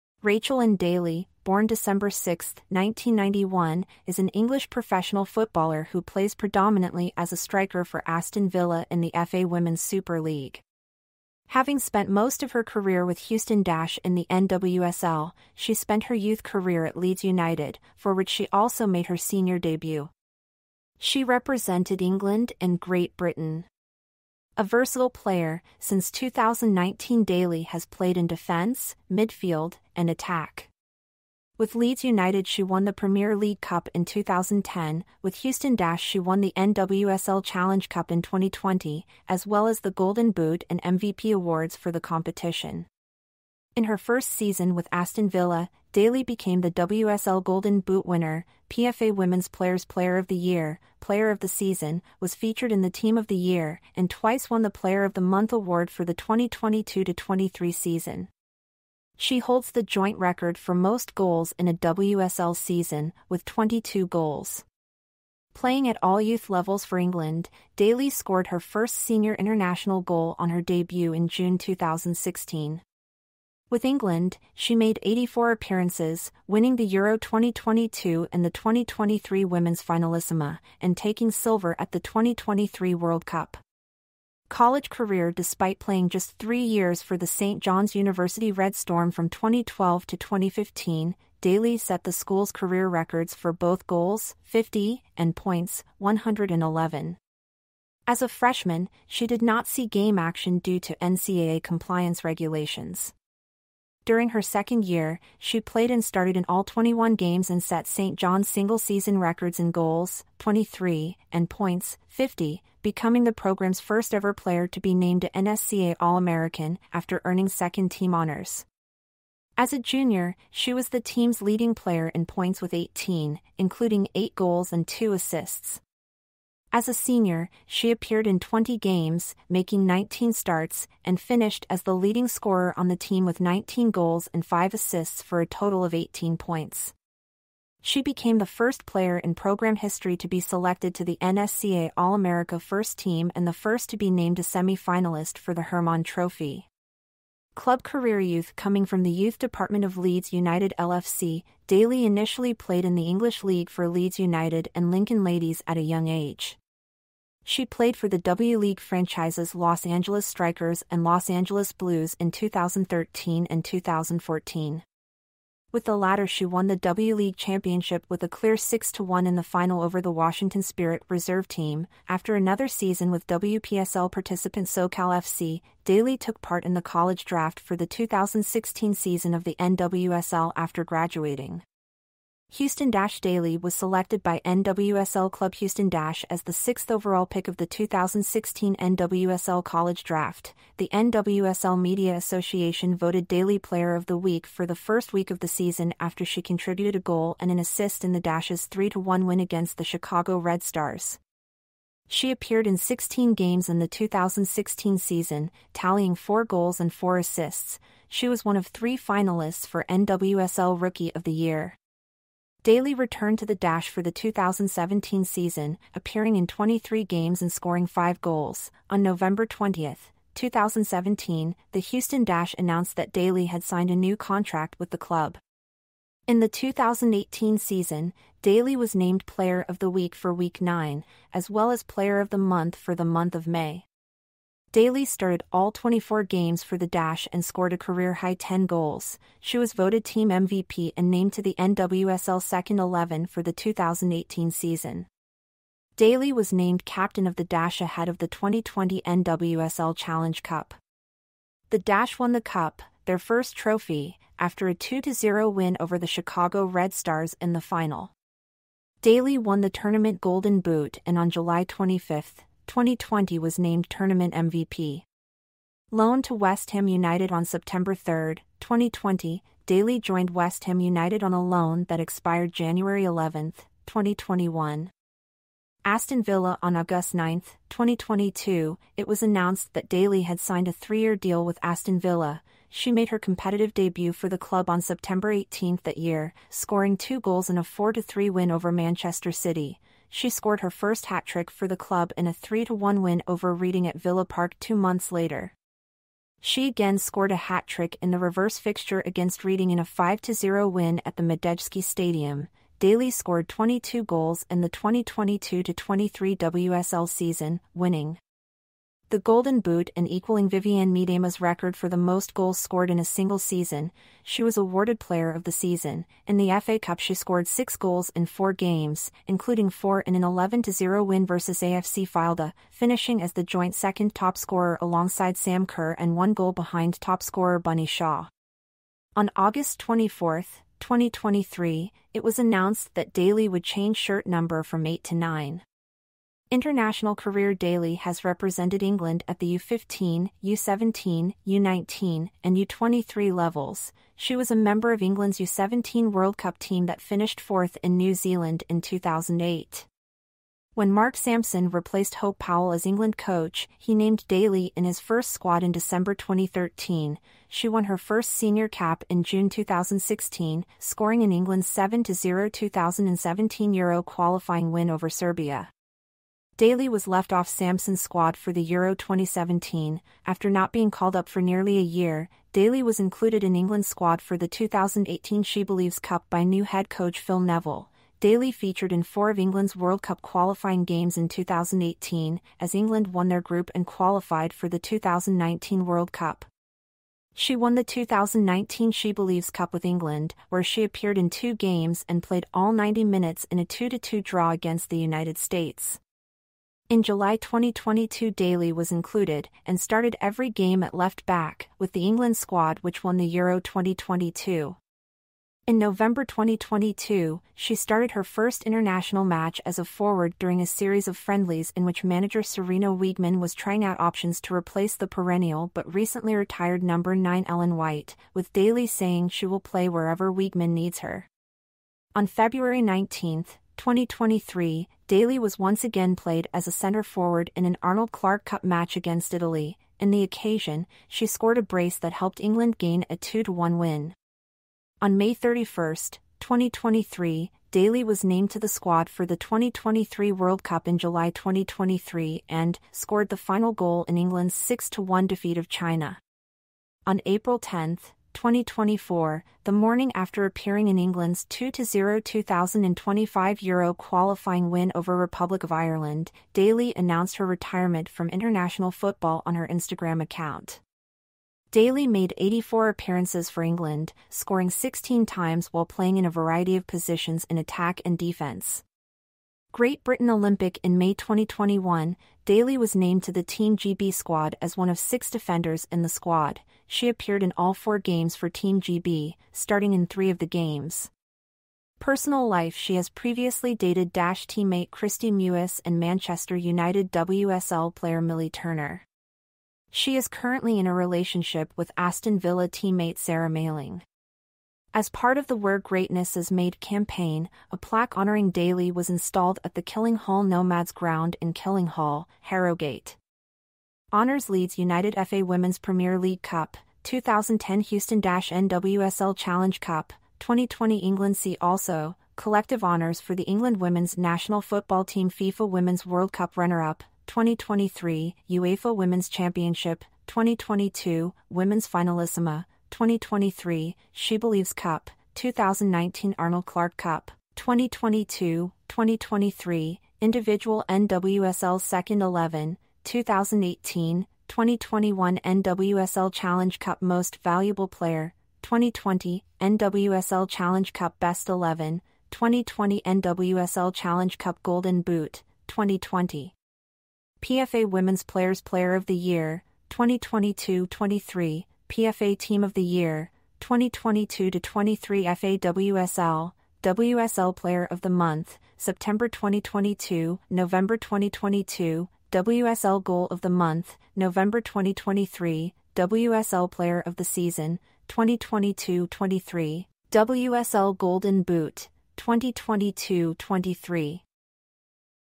The truth about Rachel Daly
Rachel Ann Daly (born 6 December 1991) is an English professional footballer who plays predominantly as a striker for Aston Villa in the FA Women's Super League. Having spent most of her career with Houston Dash in the NWSL, she spent her youth career at Leeds United, for which she also made her senior debut. She represented England and Great Britain.
A versatile player, since 2019 Daly has played in defence, midfield, and attack. With Leeds United she won the Premier League Cup in 2010; with Houston Dash she won the NWSL Challenge Cup in 2020, as well as the Golden Boot and MVP awards for the competition. In her first season with Aston Villa, Daly became the WSL Golden Boot winner, PFA Women's Players' Player of the Year, Player of the Season, was featured in the Team of the Year, and twice won the Player of the Month award for the 2022–23 season. She holds the joint record for most goals in a WSL season, with 22 goals.